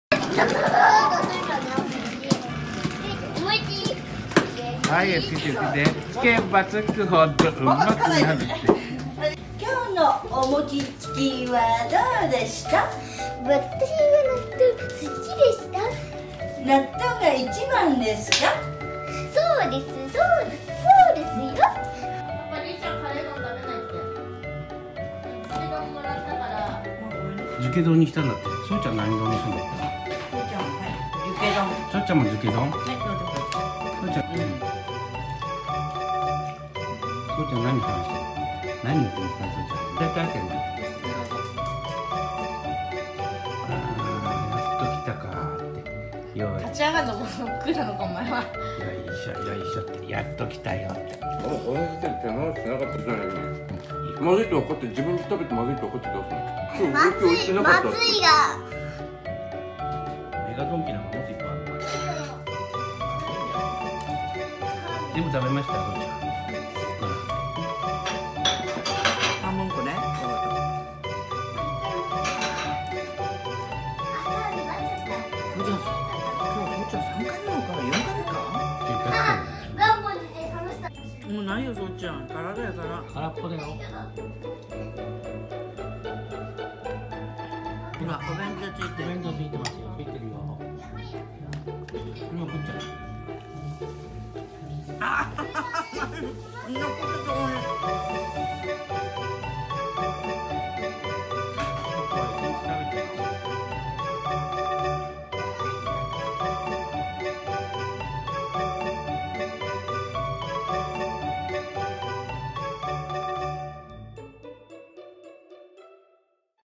秋田犬餅より鮪だよ！Akita Inu JAPANESEAKITA
grandchild and JAPANESEAKITA
秋田犬は今日も元気に過ごすそうすけ君昼間は餅つき大会でしたが参加せず、夕飯後降りてきました、何か下さい。
この日は鮪のズケ丼でしたので、僕にも下さいですね
チーズとハムも貰い、お餅は食べない秋田犬のそうすけ君
柴犬ではありません、小粒の日本犬
1years old Akita Inu JAPANESEAKITA
name"Sosuke
1years old German Shepherd Dog
name"Mac
撮影はiPhone 8 Plus
効果音
甘茶の音楽工房
ヒの字